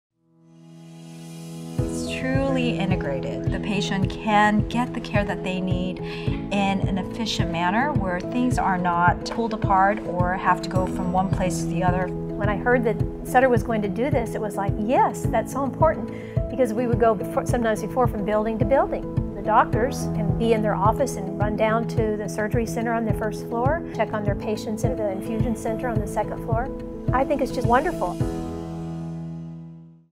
Cancer survivor describes cancer care in Roseville
At Sutter Cancer Center, Roseville, we combine advanced technology with compassionate and convenient care. Everything you and your loved ones need in one place.